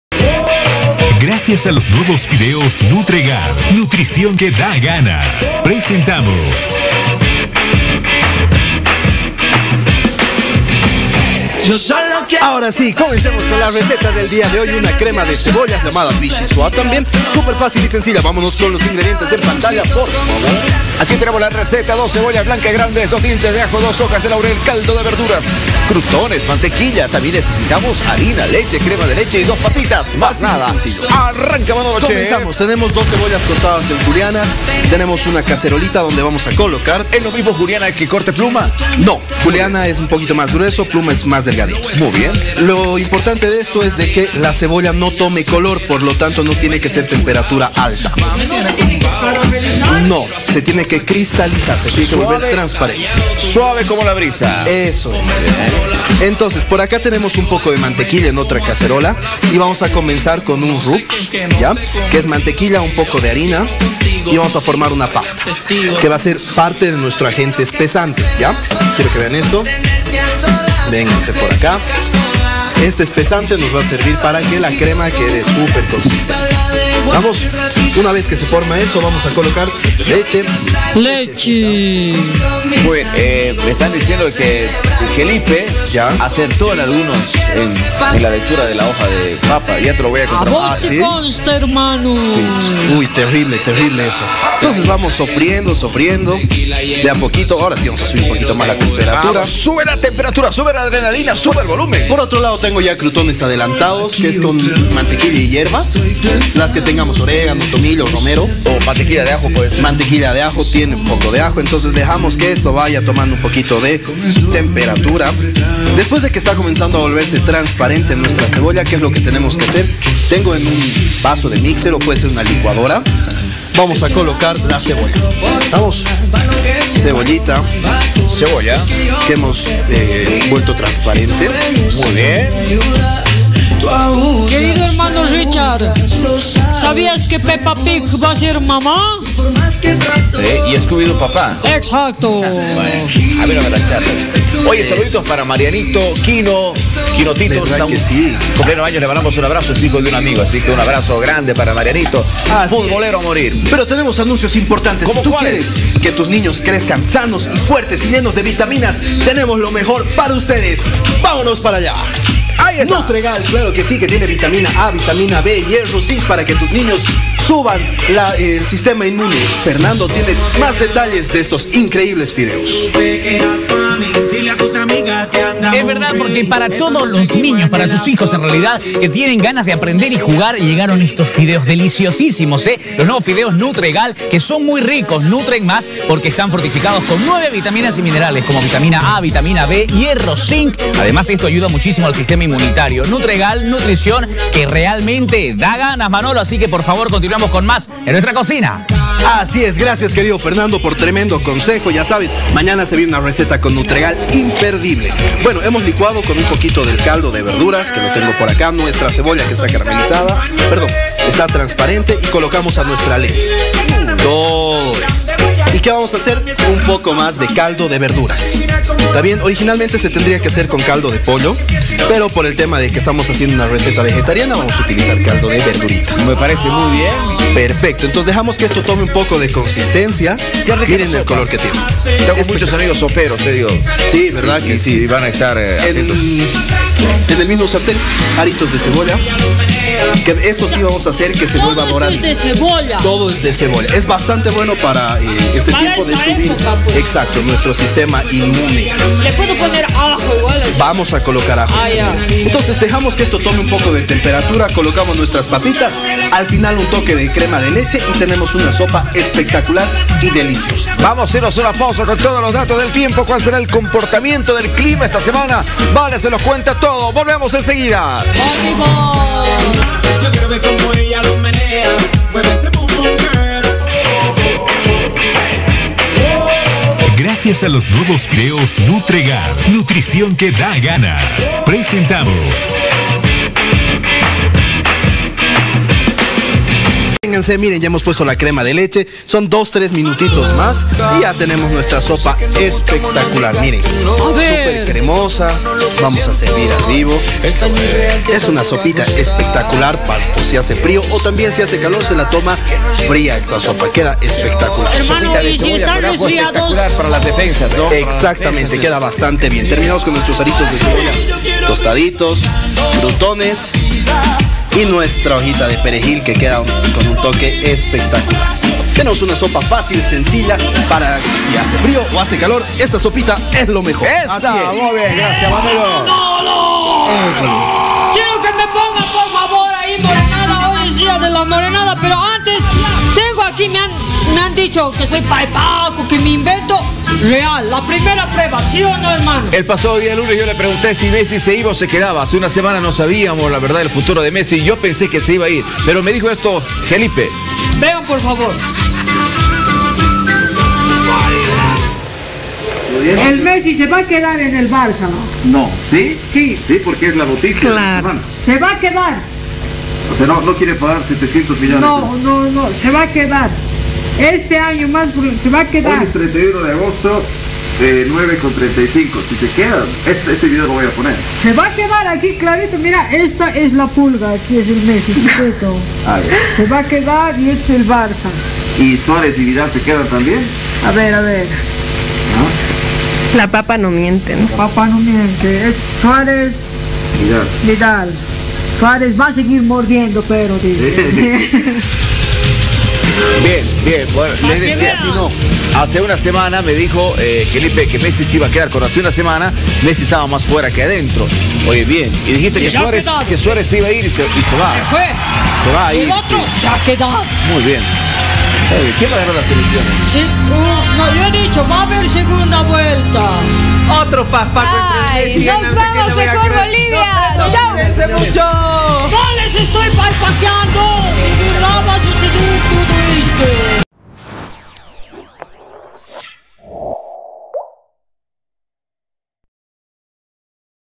Preparamos una deliciosa ''Crema de cebolla''
En el espacio de la ''Cocina al día'' le mostramos en detalle una deliciosa receta de ''Crema de cebolla'', la que puede seguir desde la comodidad de su propio hogar.